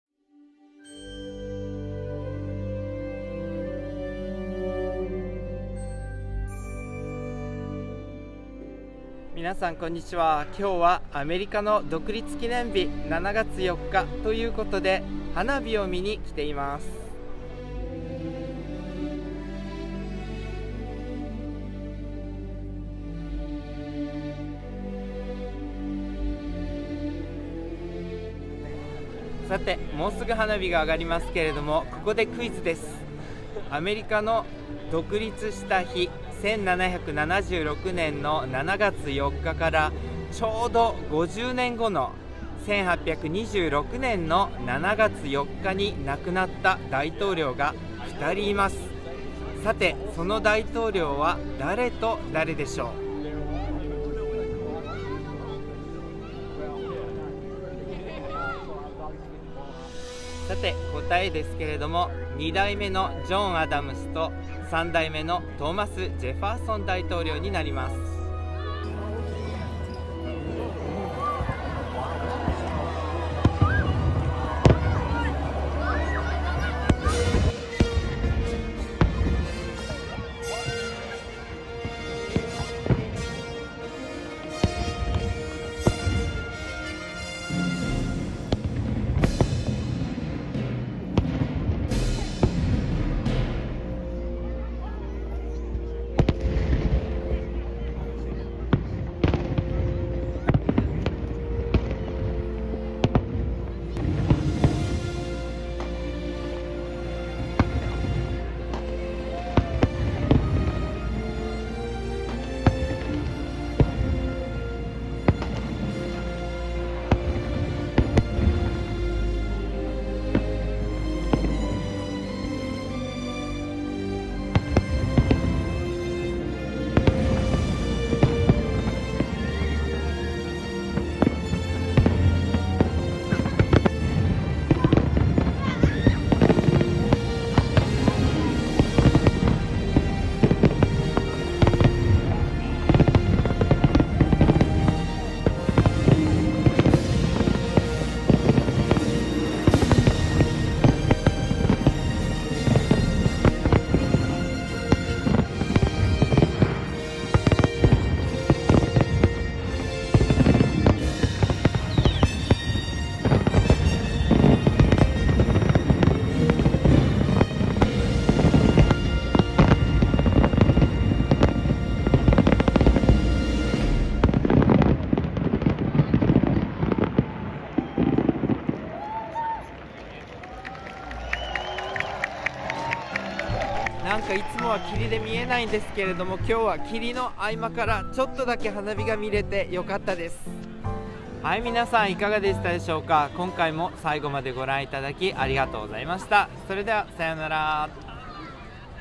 【サンフランシスコ】７月４日の独立記念日の花火は霧の中！毎年見れなくて当たり前の花火大会 July 4th Fireworks in the fog in San Francisco 2022
今回は世界三大がっかり花火大会（？）に入りそうなアメリカ合衆国の独立記念日を祝う、霧の中で炸裂するサンフランスシスコの花火大会の動画をお送りします。名物の霧が出る夏のサンフランシスコでは毎年、花火大会が霧で台無しになることが多いのですがその典型的な花火大会の動画です。霧が花火の色に染まるという変わったそれなりの美しさはありますが、やっぱり普通に花火を見たいものです。俳優のニコラス・ケージが住んでいた家の横に上がる花火も撮影しましたので最後までみてください！

This is the video of San Francisco July 4th Celebration Fireworks Show. In San Francisco, it is very common to have fireworks displays in the famous summer fog. This is how it looks. I also put the footage of the fireworks shown right next to the house, where Nicholas Cage used to live. Thank you very much for visiting my channel! Cheers! (Filmed at Francisco Park, San Francisco July 4th, 2022)